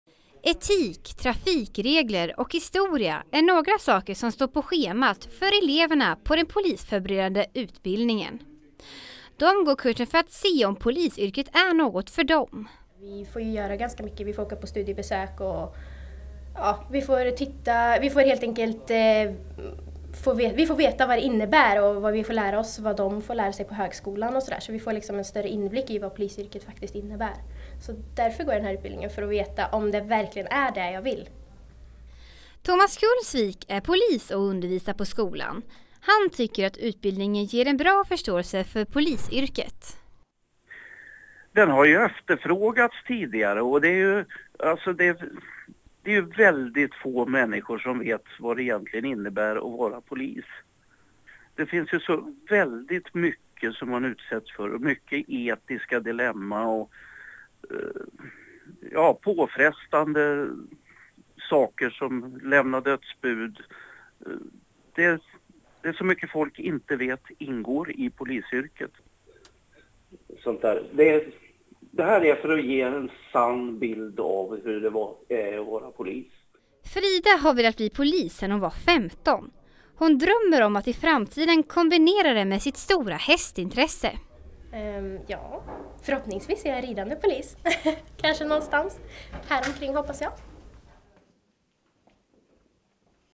Tema rättsväsende: Den polisförberedande utbildningen i Hjo
Att bli polis är en barndomsdröm för många. Men om man är osäker på om det är rätta yrket för en, kan man gå en polisförberedande utbildning på folkhögskolan i Hjo.
Ett tv-reportage av Astrid Jensen på Vara folkhögskolas radiolinje.